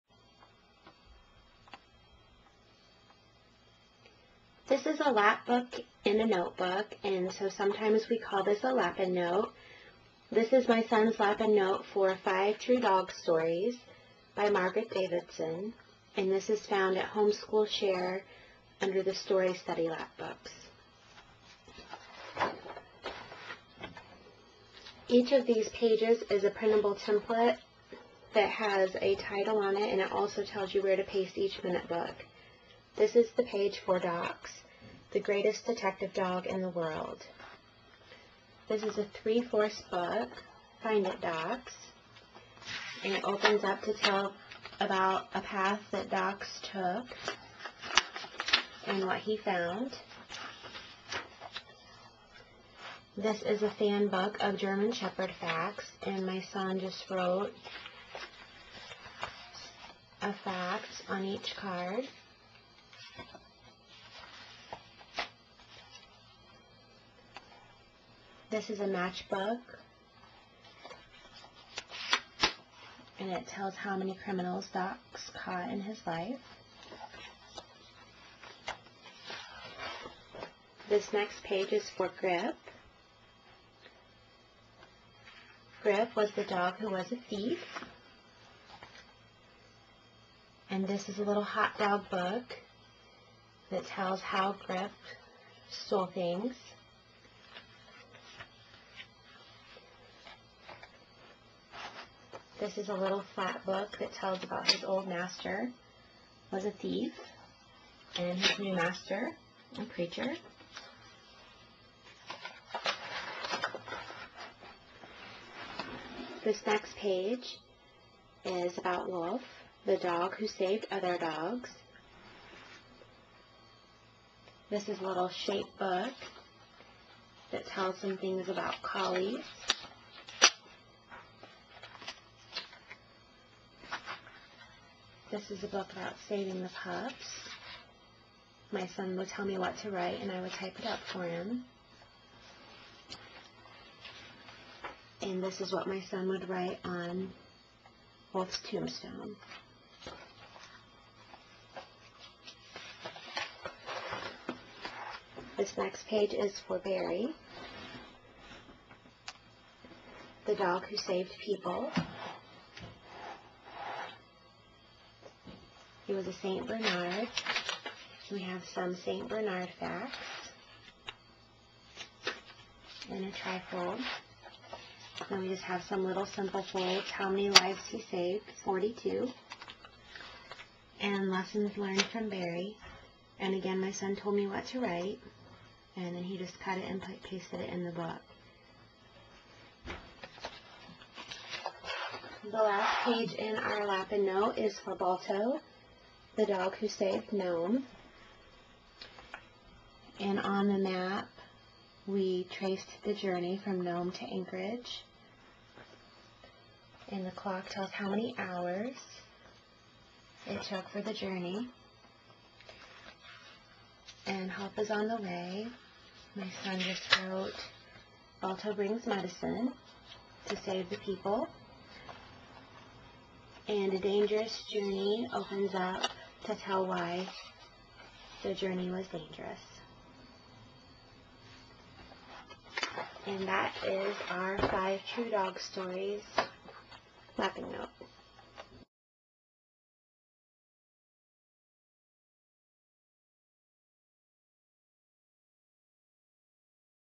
HSS Lapbook - Five True Dog Stories
This is a book report lapbook my son did for first grade.